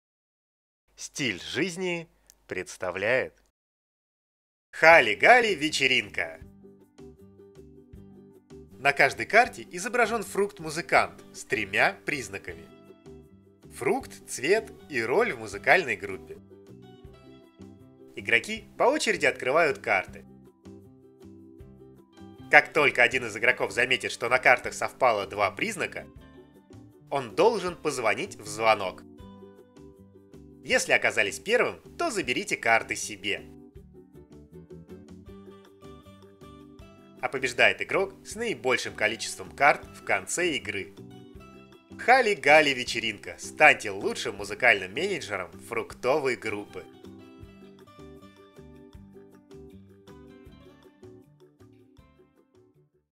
Халли Галли Вечеринка. Обзор настольной игры от компании Стиль Жизни
А знаете ли вы, что фрукты тоже умеют веселиться? В настольной игре Халли Галли Вечеринка они создали свою музыкальную группу и устроили настоящий концерт!

Ищите совпадающие признаки, вовремя бейте по звонку и станьте лучшим музыкальным менеджером фруктовой группы!

«Стиль жизни» — издательство настольных игр и оптовая компания, свою первую настольную игру мы издали в 2007 году, а первым дистрибьютором стали в 2008 году.